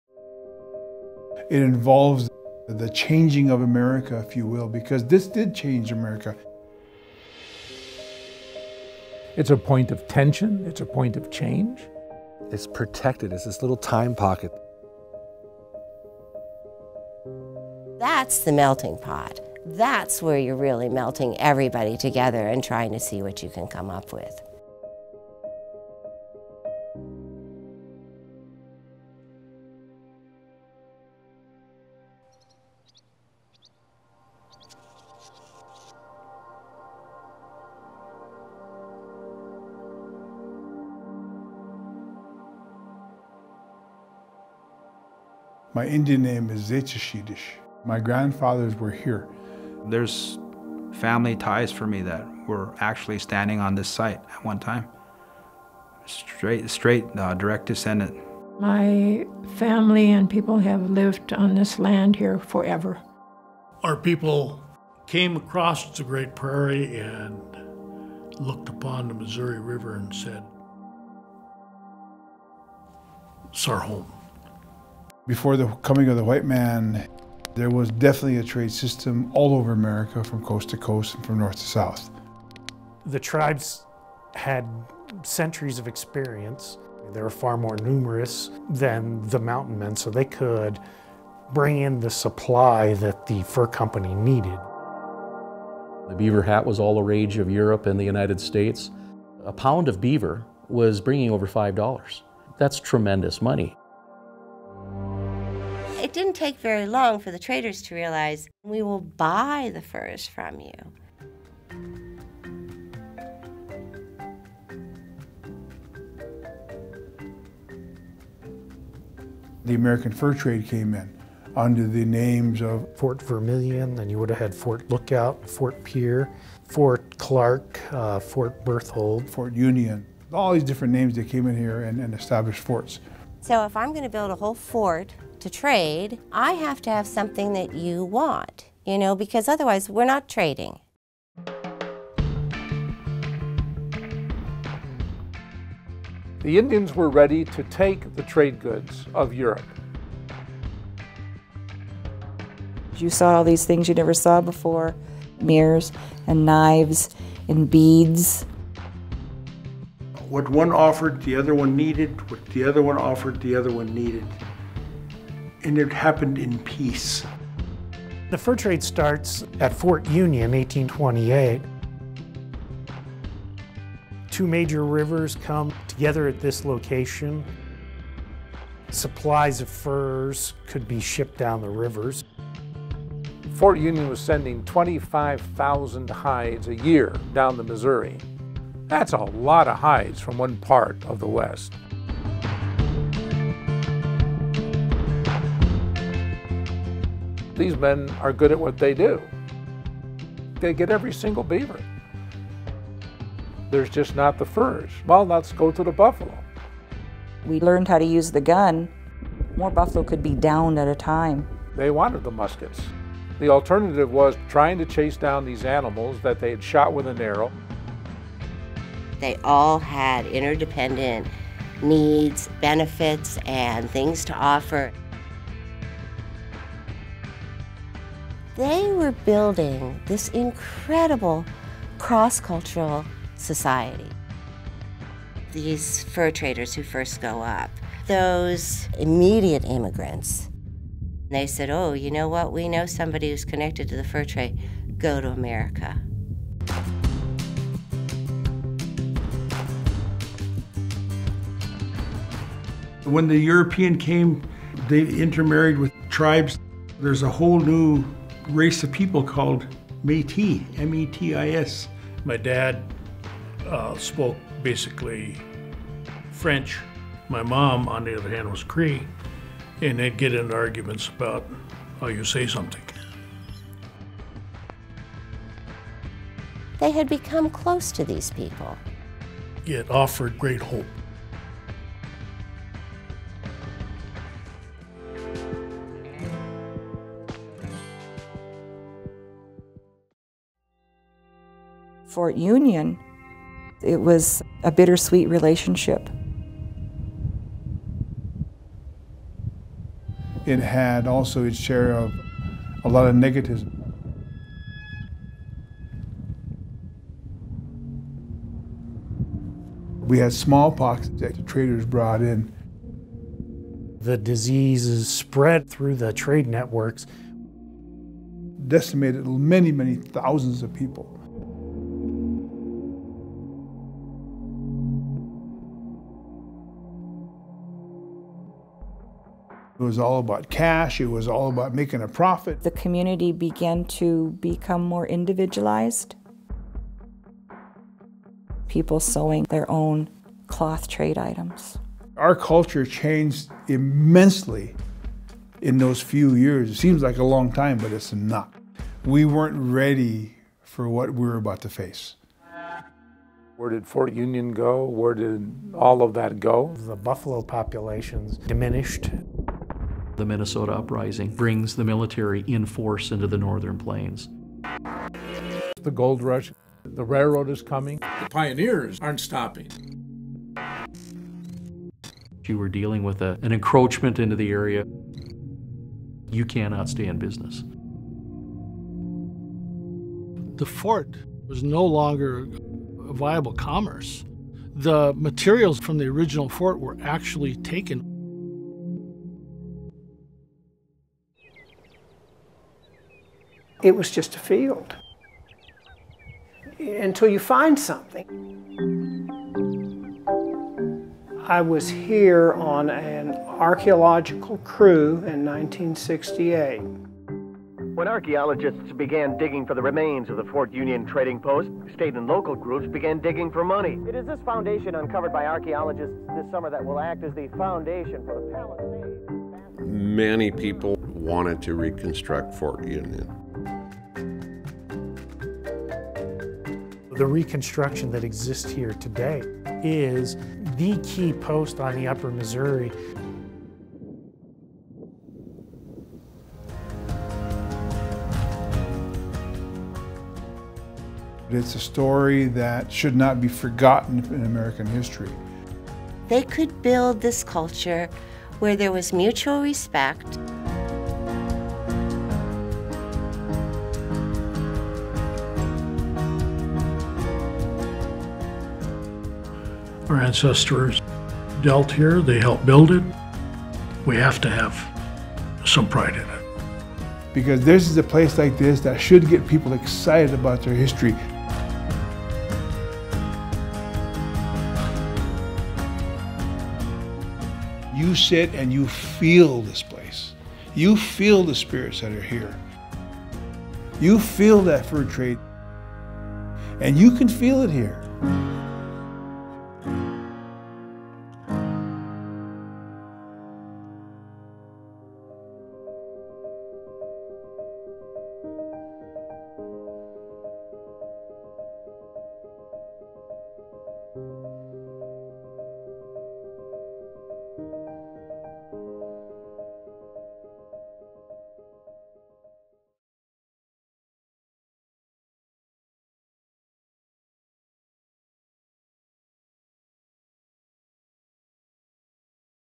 WATCH: Historical Fort Has Amazing Fur Trade History
Fort Union Trading Post was an epicenter for trade between tribes and European fur traders. Located at the confluence of the Yellowstone and the Upper Missouri river, the fort served as the premier trading post for beaver pelts and buffalo robes.